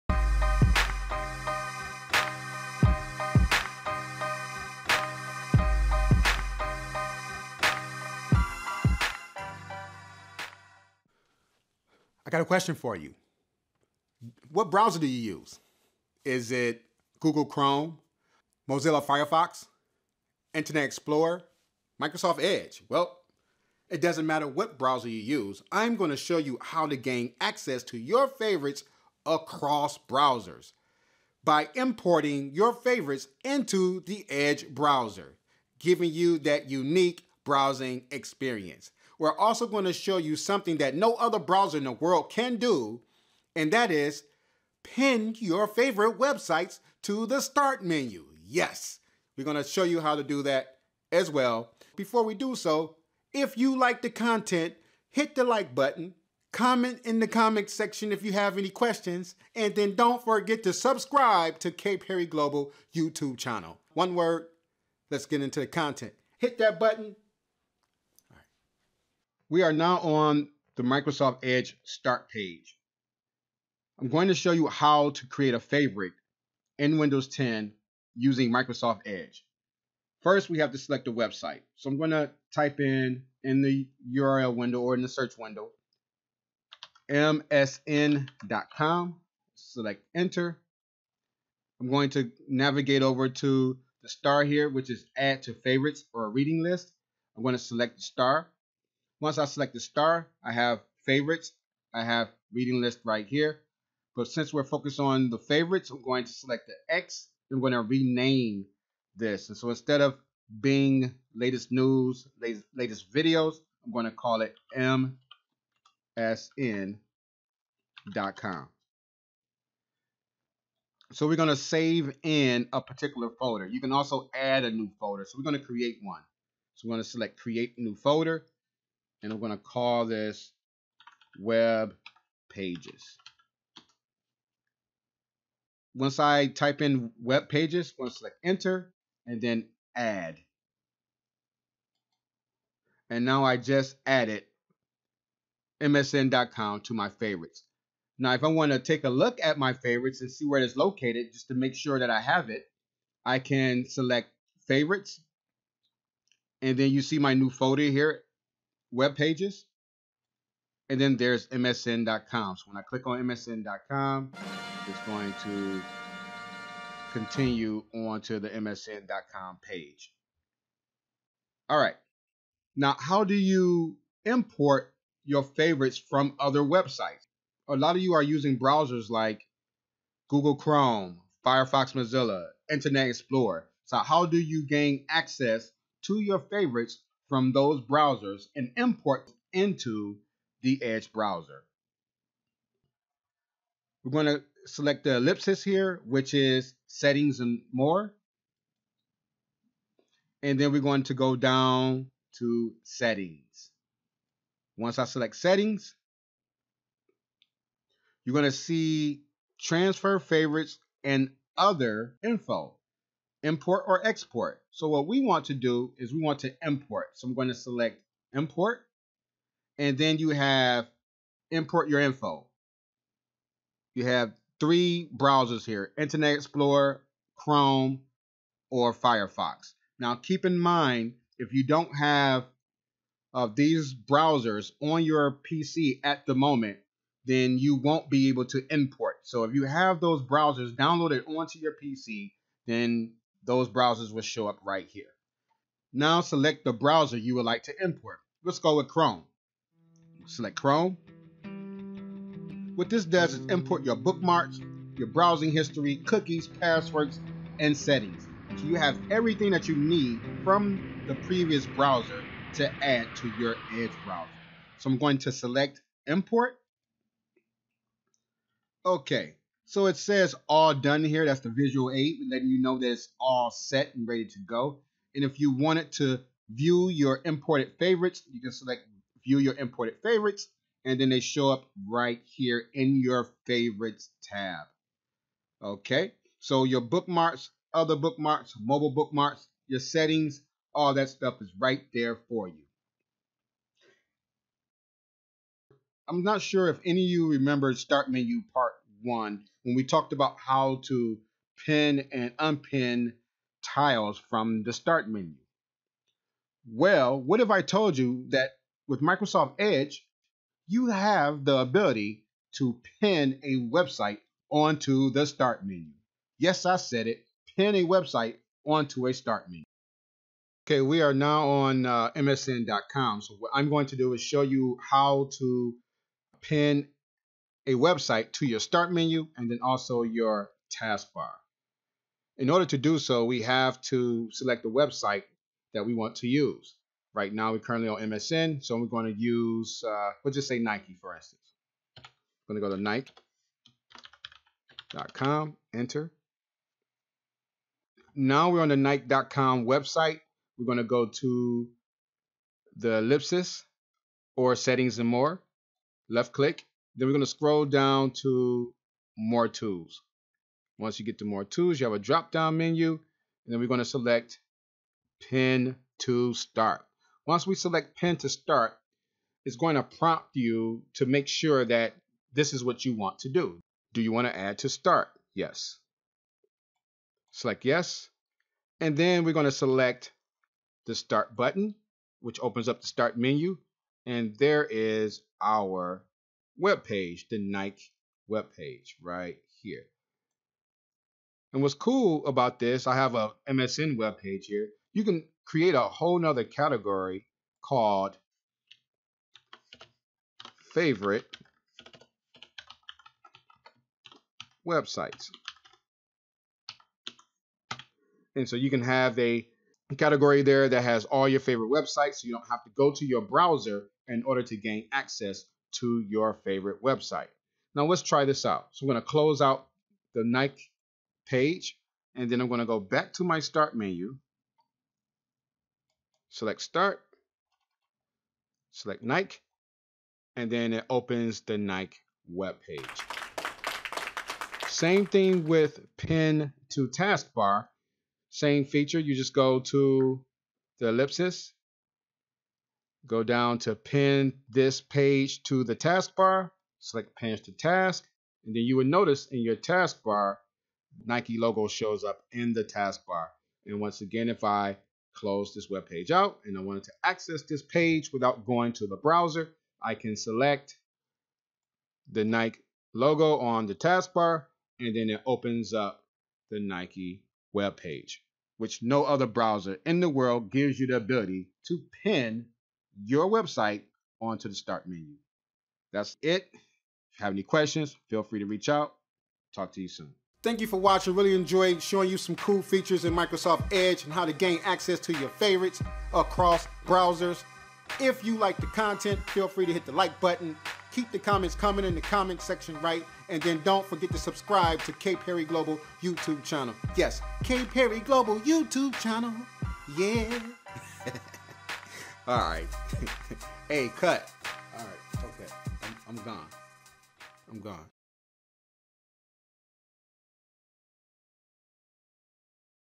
How To Save And Import Favorites In Microsoft Edge Browser
Explore how to create and import your Favorites into the Microsoft Edge browser.  Learn how to Pin websites onto the Start Menu, which gives you the ability to launch a website with a click of a button.